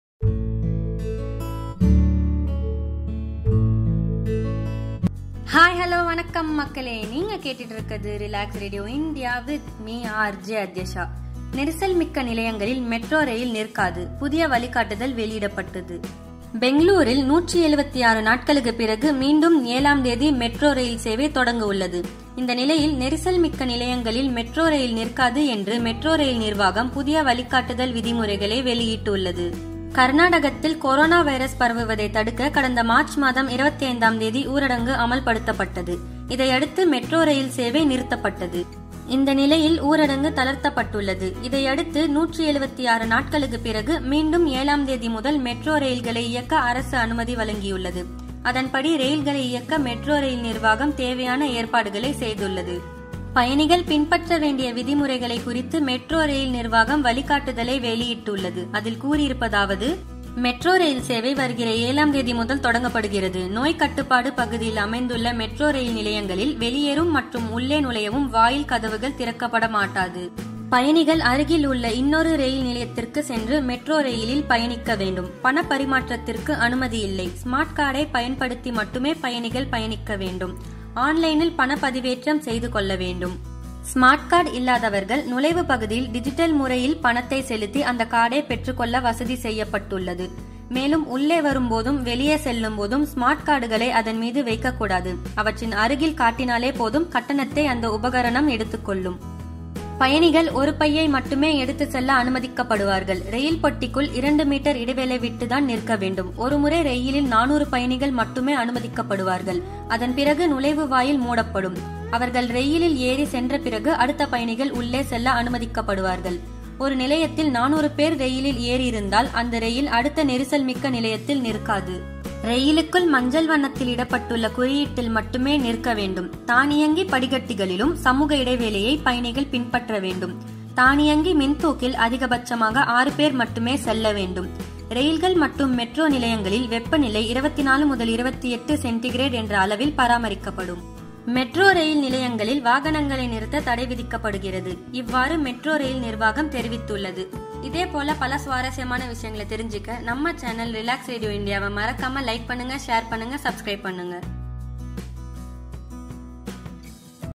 RelaxRadioIndia | நெரிசல் மிக்க நிலையங்களில் மெட்ரோ ரெயில் நிற்காது | Adhyasha
RelaxRadioIndia | நெரிசல் மிக்க நிலையங்களில் மெட்ரோ ரெயில் நிற்காது: புதிய வழிகாட்டுதல் வெளியிடப்பட்டது | Adhyasha

Metro Rail will not stop at congested stations: New guideline issued

Editor : Amuthan